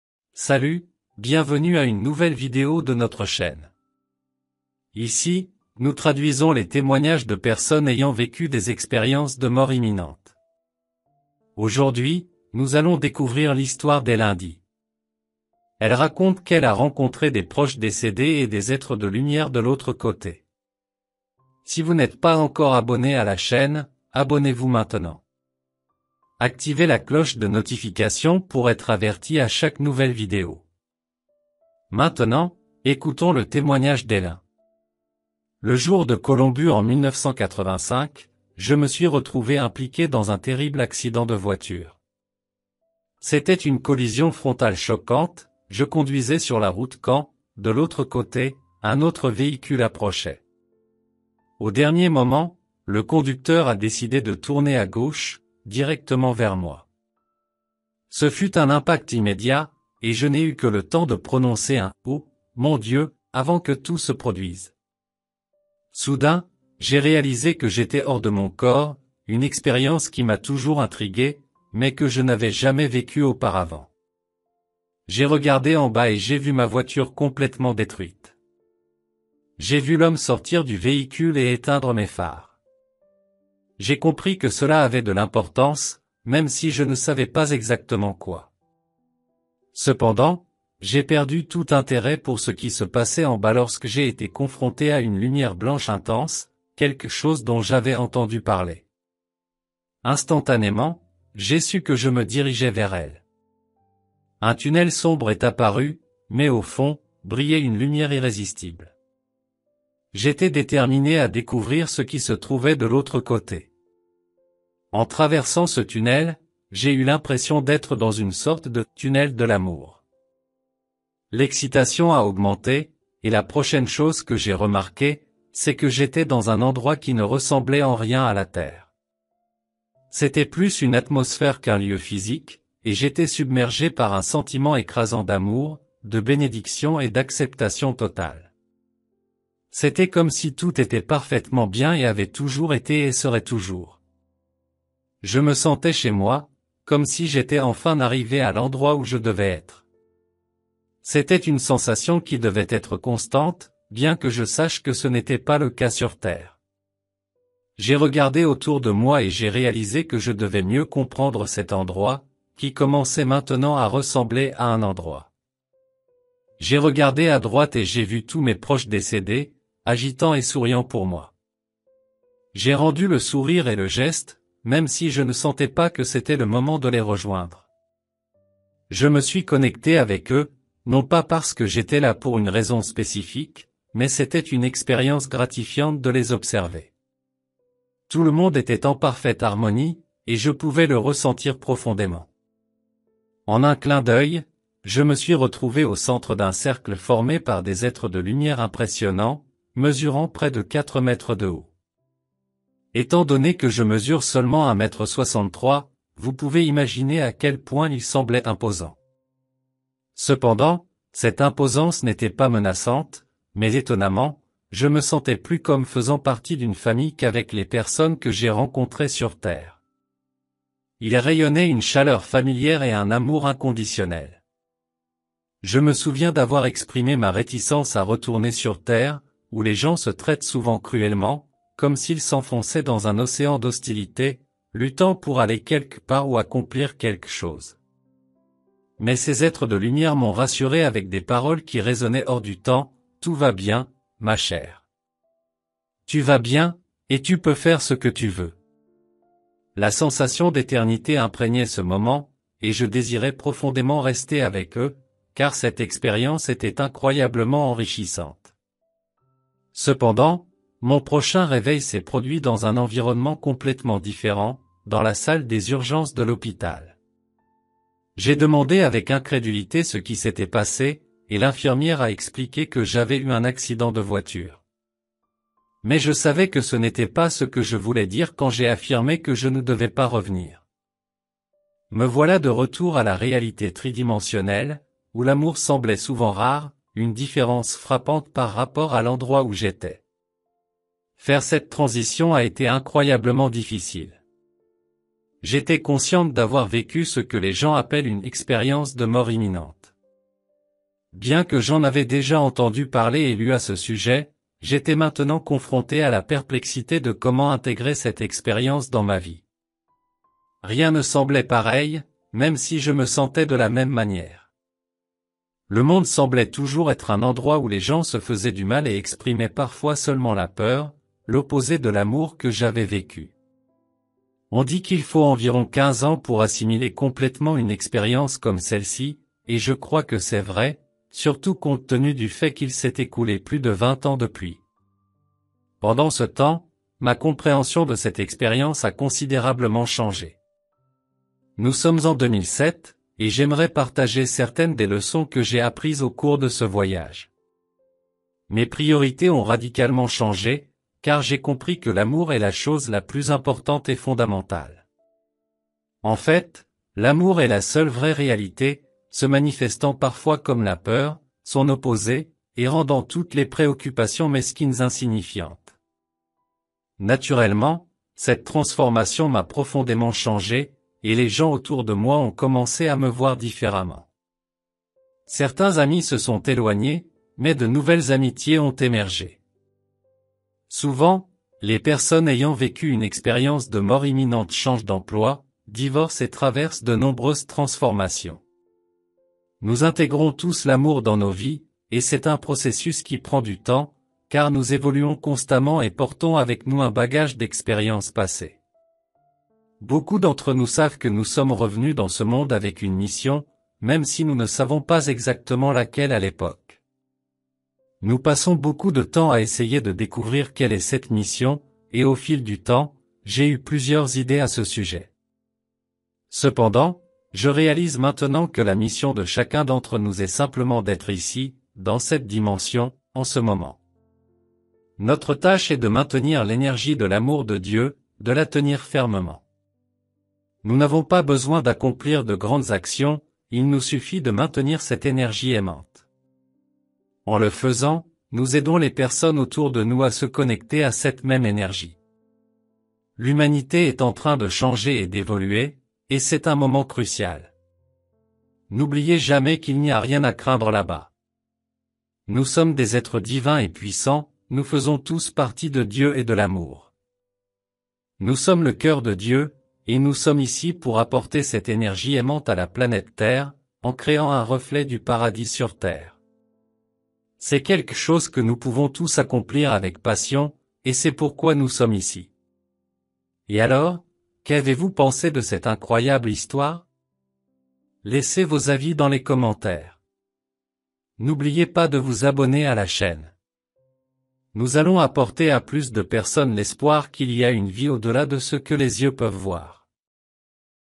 EMI : Elle a rencontré des proches décédés et d'autres êtres - Expérience de mort imminente
Demandez à tenir la chaîne, qui est la quantité.

EMI : aujourd'hui, nous allons connaître l'histoire d'Ellyn Dye. Elle dit avoir trouvé des parents morts et des êtres de lumière de l'autre côté.